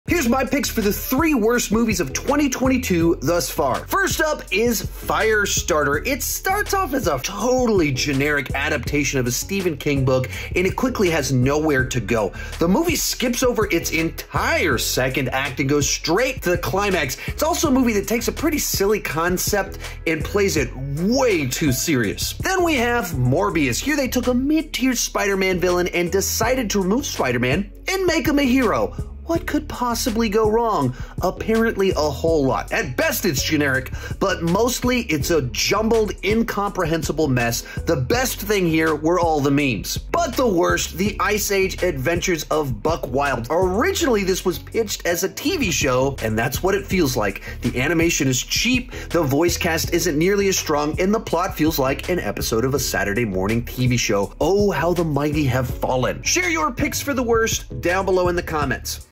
Worst Movies of 2022...Thus Far!
What are your picks for the worst movies of 2022...thus far!

VIDEO SUMMARY
This video contains Sean Chandler Talks About's Sean Chandler movie reviews worst movies of 2022...thus far!  /// Firestarter review /// morbius review